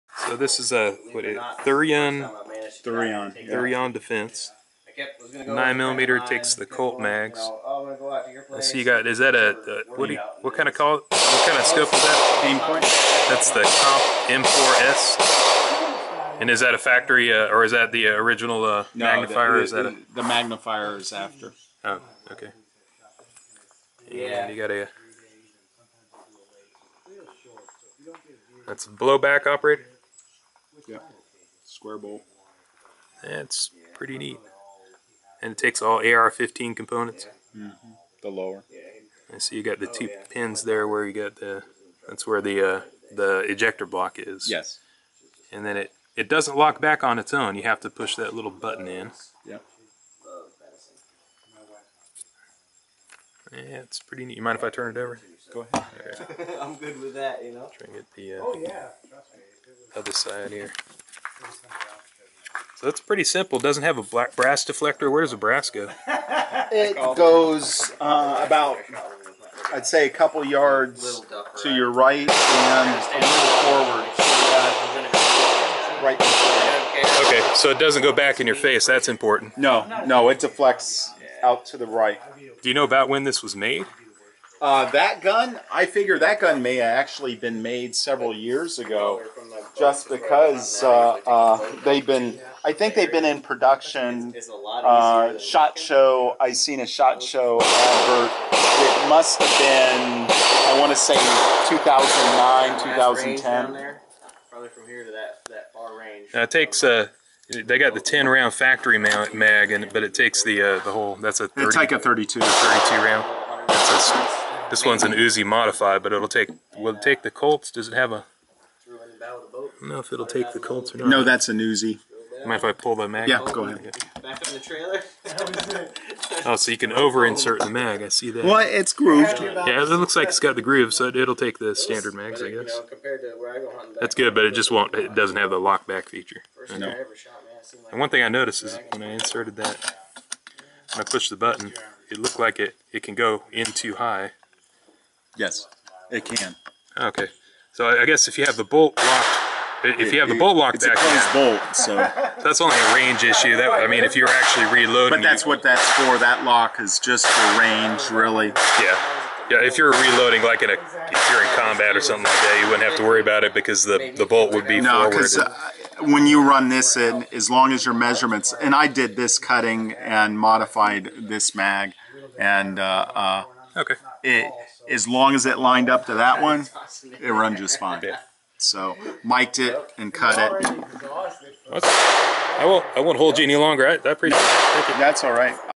Thureon Defense SA 9mm Carbine
At the range and a fellow club-member was shooting this carbine.  I thought at a glance that he was shooting the "Just Right Carbine" which I've found to be a piece of junk.  I had my Kel Tec Sub 2000 out so I struck up a conversation and he let me film it.

As I did not get to shoot it, I'll give you my general impressions in writing:

Pros:
1) Easy to take down and maintain
2) Solid, good quality build
3) Reliable
4) Available in 9mm, 40, .45, 10mm, and with Glock or Colt mags

Cons:
1) No bolt hold-open
2) Rail appears too high above stock line
3) I don't have one (YET!)
4) How do you pronounce Thureon?